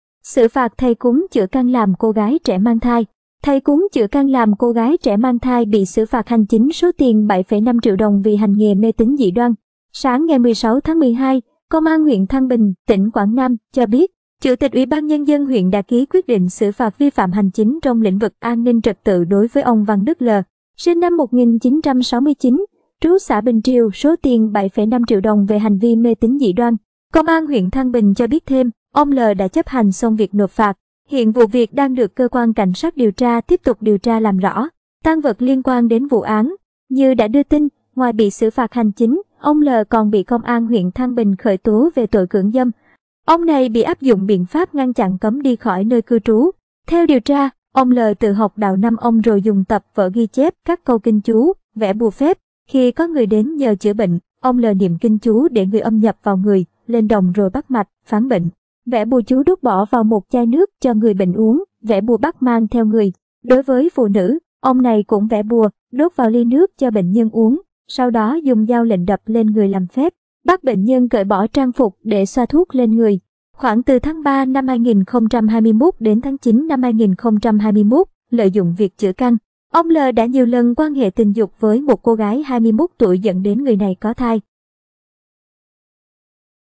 Xử phạt thầy cúng chữa 'căn' làm cô gái trẻ mang thai
Thầy cúng chữa 'căn' làm cô gái trẻ mang thai bị xử phạt hành chính số tiền 7,5 triệu đồng vì hành nghề mê tín dị đoan.Tang vật liên quan đến vụ án
Khi có người đến nhờ chữa bệnh, ông L. niệm kinh chú để “người âm” nhập vào người, lên đồng rồi bắt mạch, phán bệnh, vẽ bùa chú đốt bỏ vào một chai nước cho “người bệnh” uống, vẽ bùa bắt mang theo người.
Như đã đưa tin, ngoài bị xử phạt hành chính, ông L. còn bị Công an huyện Thăng Bình khởi tố về tội “Cưỡng dâm”. Ông này bị áp dụng biện pháp ngăn chặn cấm đi khỏi nơi cư trú.
Đối với phụ nữ, ông này cũng vẽ bùa, đốt vào ly nước cho bệnh nhân uống, sau đó dùng “dao lệnh” đập lên người làm phép, bắt bệnh nhân cởi bỏ trang phục để xoa thuốc lên người.
Khoảng từ tháng 3/2021 đến tháng 9/2021, lợi dụng việc chữa “căn”, ông L. đã nhiều lần quan hệ tình dục với một cô gái 21 tuổi dẫn đến người này có thai.
Công an huyện Thăng Bình cho biết thêm, ông L. đã chấp hành xong việc nộp phạt. Hiện vụ việc đang được Cơ quan Cảnh sát điều tra tiếp tục điều tra làm rõ.
Theo điều tra, ông L. tự học đạo “Năm Ông” rồi dùng tập vở ghi chép các câu kinh chú, vẽ bùa phép.
Sáng 16/12, Công an huyện Thăng Bình (tỉnh Quảng Nam) cho biết, Chủ tịch UBND huyện đã ký quyết định xử phạt vi phạm hành chính trong lĩnh vực an ninh trật tự đối với ông Văn Đức L. (SN 1969, trú xã Bình Triều) số tiền 7,5 triệu đồng về hành vi mê tín dị đoan.